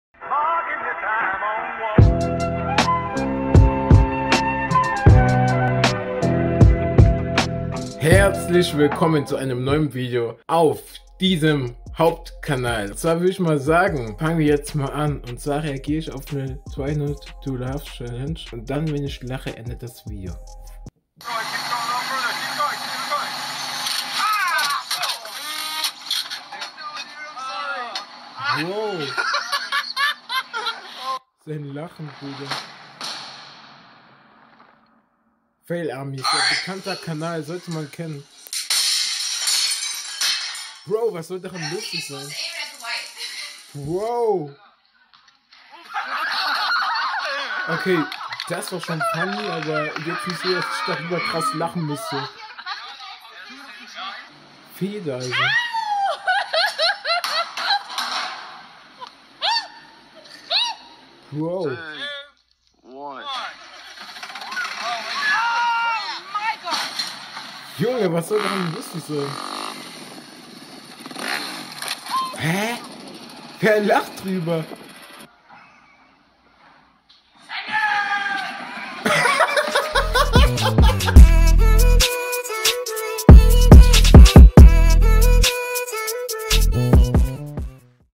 1x LACHEN = VIDEO ZU ENDE!😂 | Try Not to Laugh
In diesem Video mache ich eine "Try Not to Laugh" Challenge was bedeutet,dass ich nicht lachen darf. Sobald ich lache ist das Video vorbei.